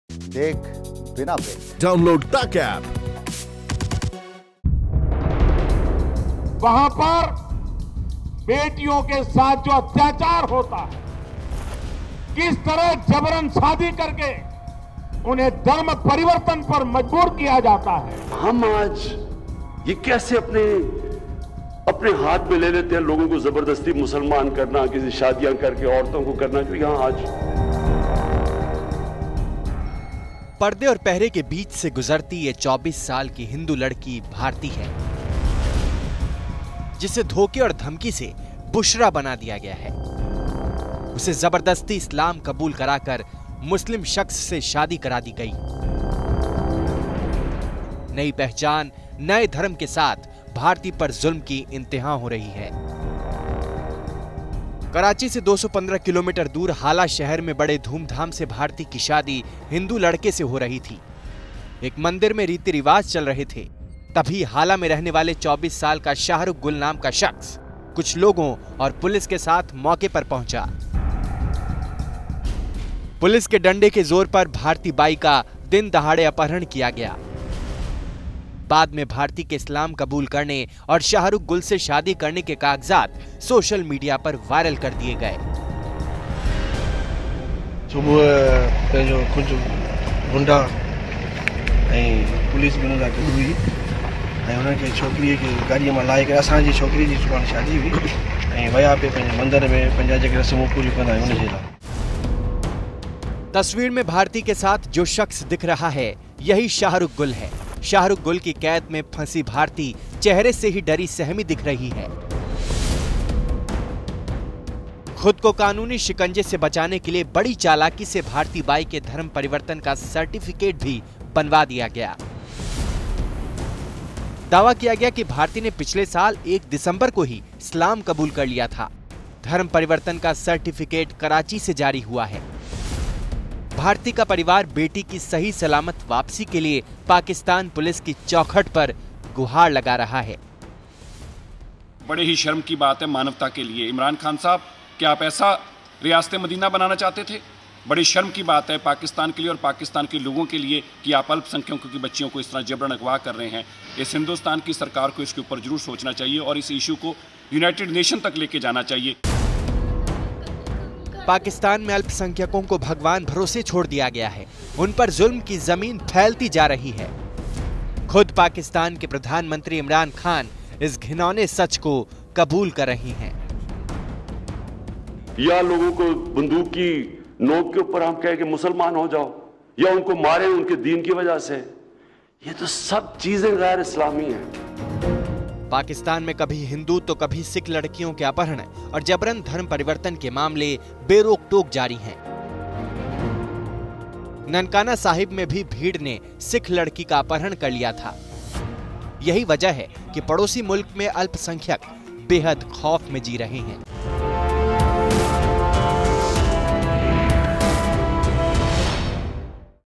देख लो इमरान, अल्पसंख्यकों पर अत्याचार, पाकिस्तान में ये क्या हो रहा है?
साल 2020 पाकिस्तान में हिंदू समुदाय के लिए कठिनाइयों का साल बन गया है. वहां जबरन अपहरण, धर्म परिवर्तन और मुस्लिम पुरुषों से शादी आए दिनों की बात हो गई है. कराची से करीब 215 किलोमीटर दूर मटियारी जिले में स्थित हाला शहर में हिंदू युवती के धर्मपरिवर्तन और जबरन शादी का एक और मामला सामने आया है. युवती को स्थानीय पुलिस अधिकारियों की देखरेख में हमलावरों ने विवाह समारोह से किडनैप किया और फिर जबरन इस्लाम कबूल करवाकर एक मुस्लिम व्यक्ति से उसकी शादी कर दी गई.